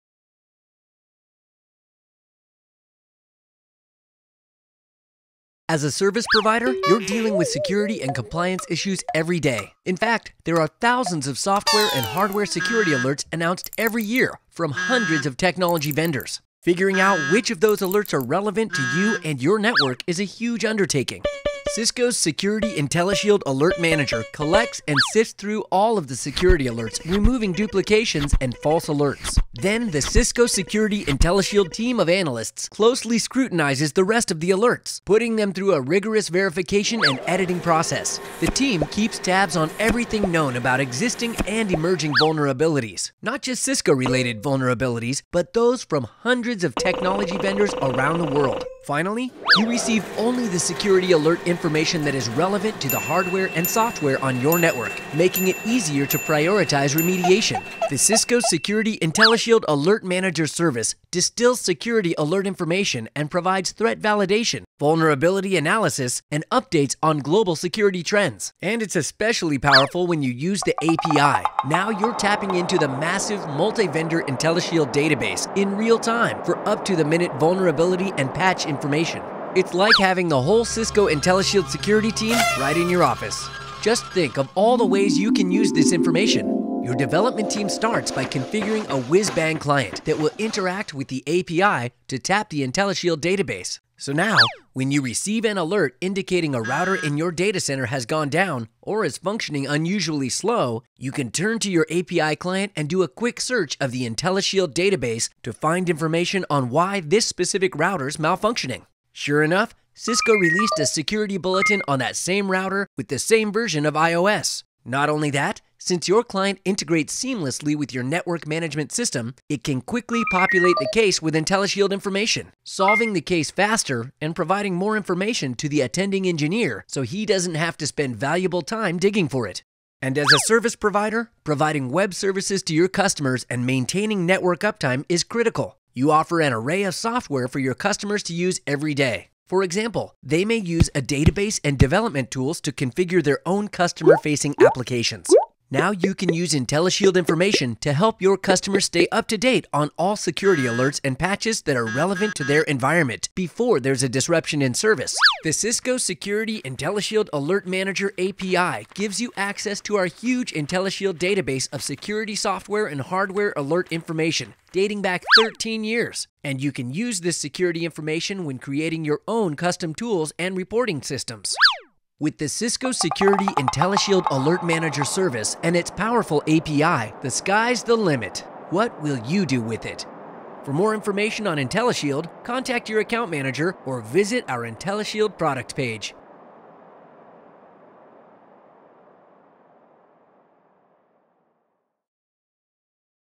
Cisco Security IntelliShield Alert Manager API
The Cisco Security IntelliShield Alert Manager
Service filters through the multitude of alerts from
reporting organizations to provide the strategic,
targeted security intelligence customers can use
to proactively respond to potential IT threats,
mitigate risk, and increase business continuity.